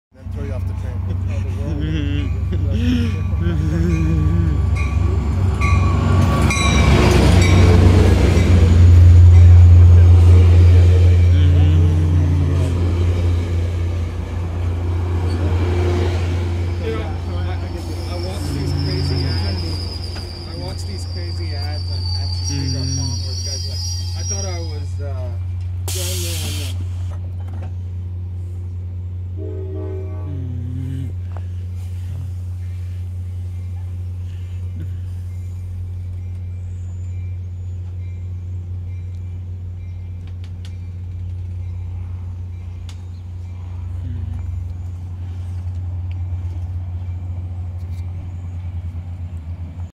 Ace 3101 8:18 arriving and leaving pleasanton
Ace 3101 leads ace #7 in and out of pleasanton